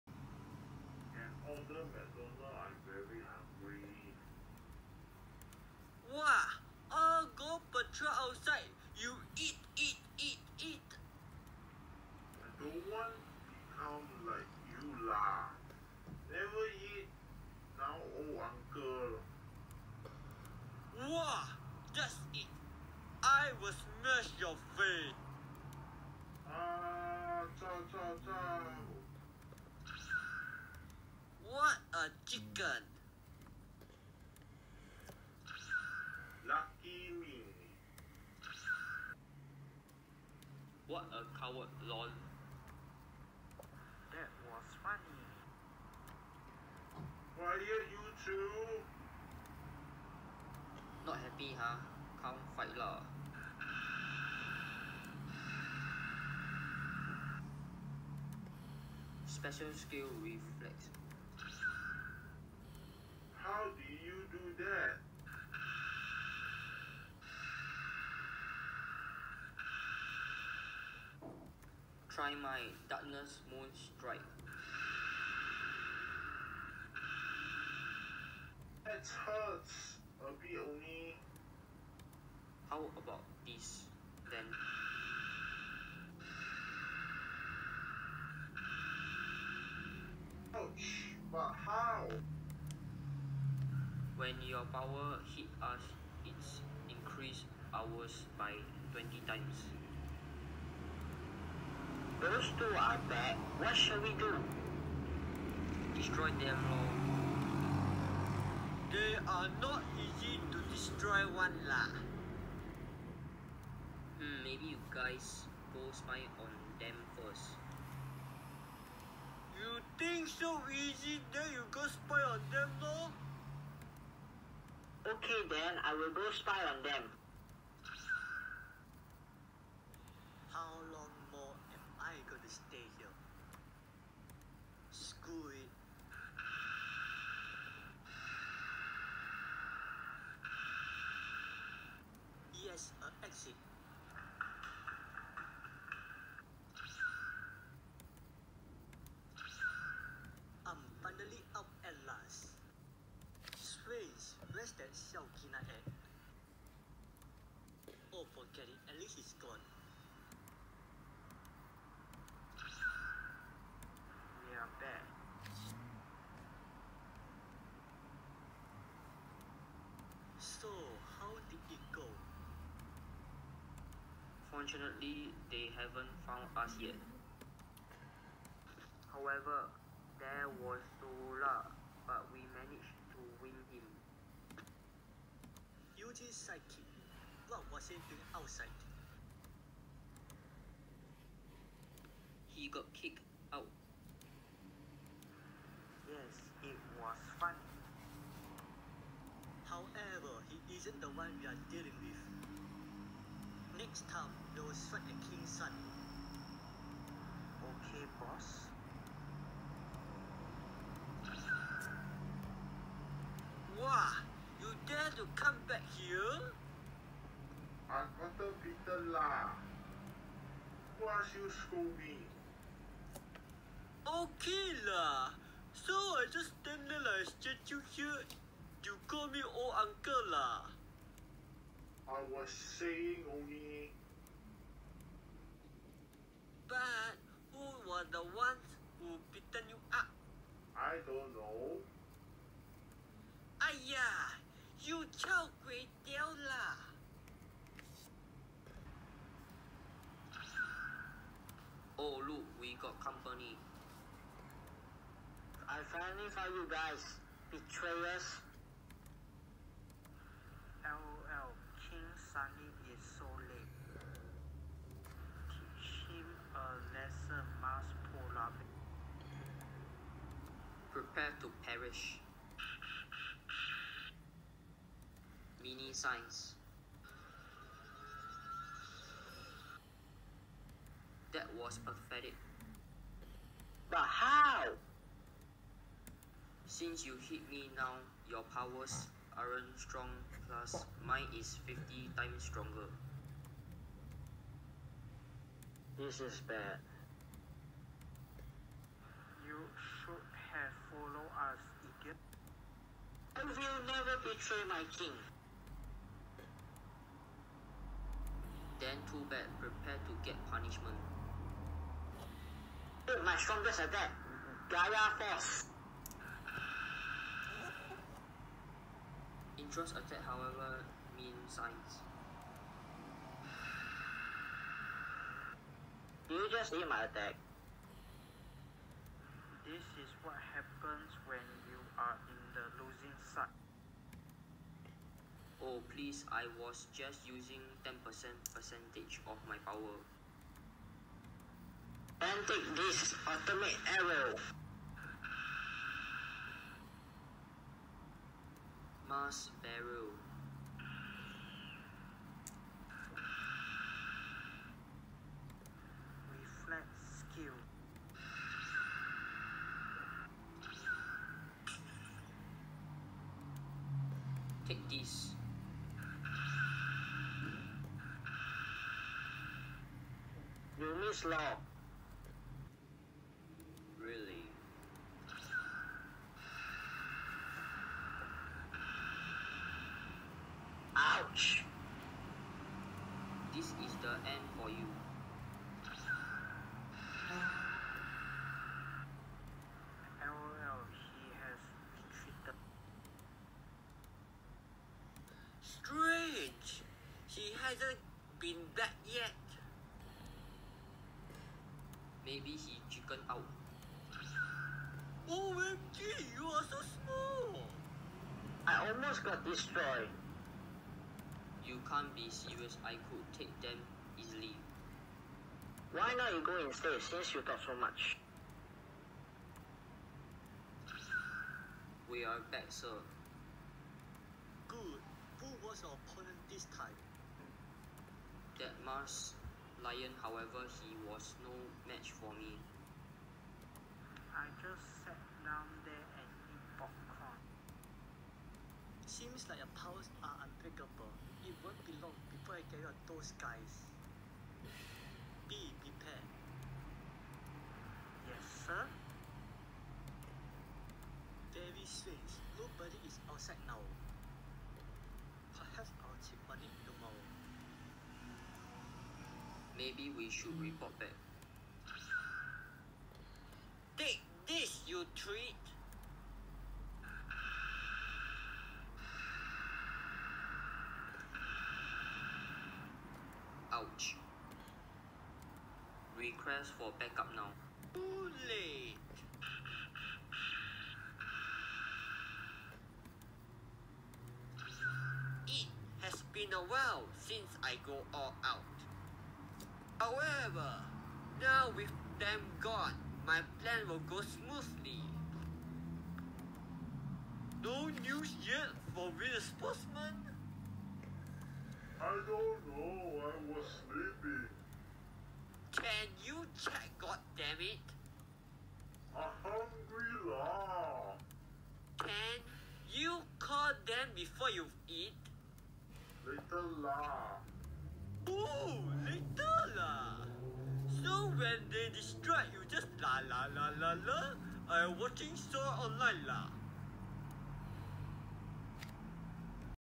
Five New Heroes 2020 Remastered Sonix’s Pov episode 6 King Jackson’s Followers
Casts
Sonix as himself
Rookie as Joe
Mars Polar Bear as Brandon
Hero Solar as Prince
President UG: Jun
King Jackson as Sonix
? As Jun